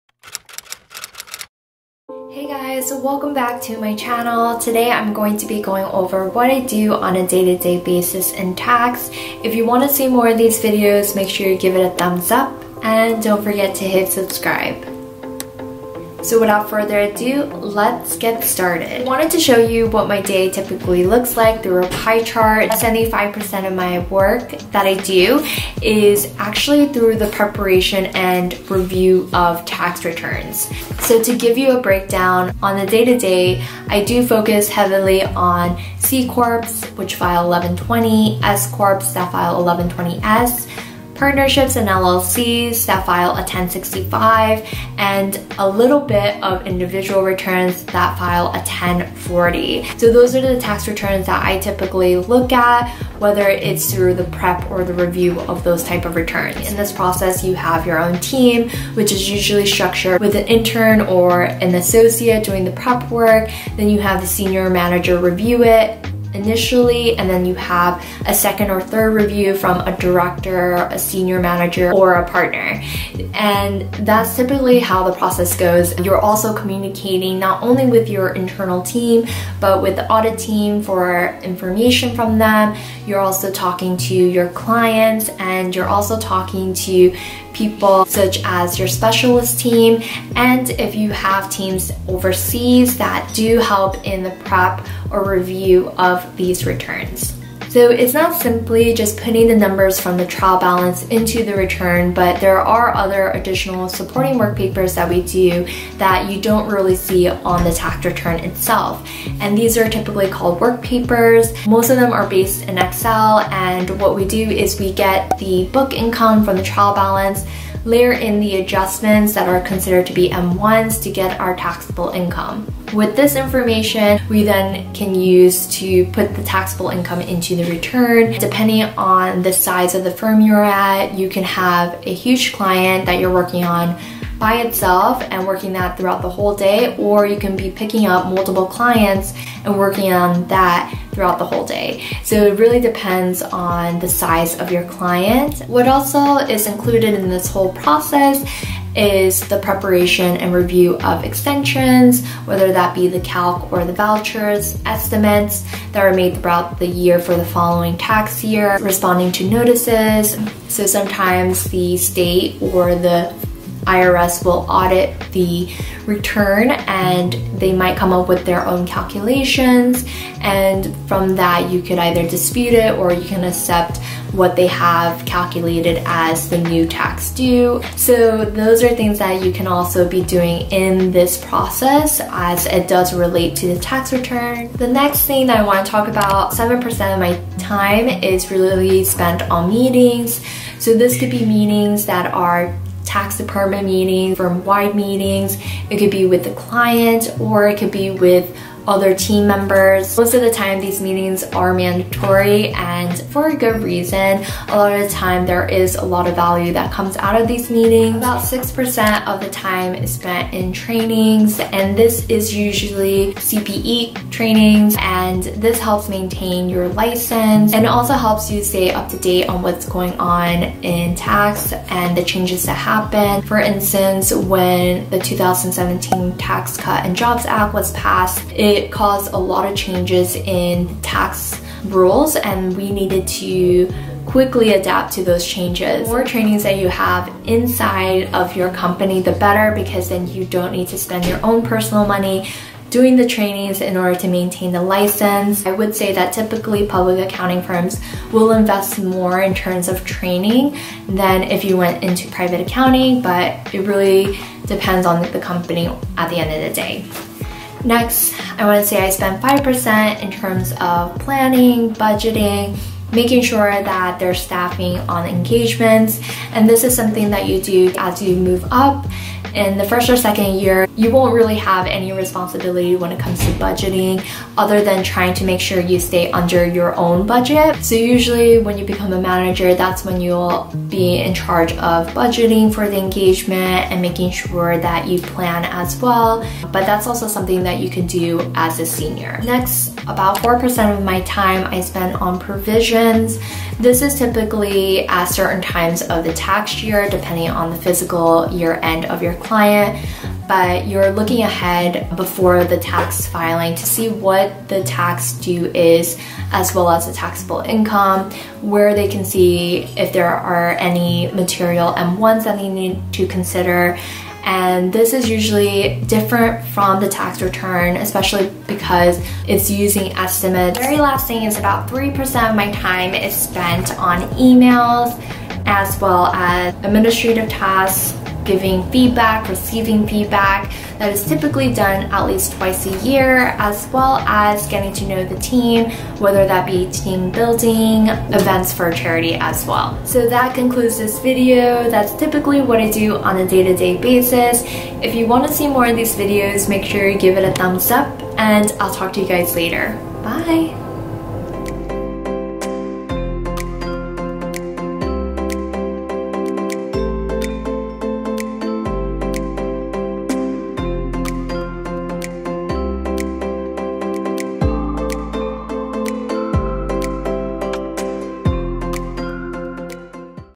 What do Tax Accountants do? l Day in a Life of a Tax Accountant l What you Should Expect
Hey guys!

In this video, I discuss what I do as a tax accountant/ manager. I've been in the accounting industry for 5 years+ and I hope this video helps both current & future accountants.

Different Topics discussed in this video:
1. Preparing / Reviewing Tax Returns
2. Book vs Taxable Income
3. Meetings
4. Trainings
5. Budgeting/ Planning/ Staffing
6. Provisions
7. E-mails, Admin, Feedback
8. Company Events

Thanks!
Jen

📷  Equipment Used:
(Amazon Affilate Links below)
Sony Alpha a6000 Camera
Amazon Basics Lightweight Tripod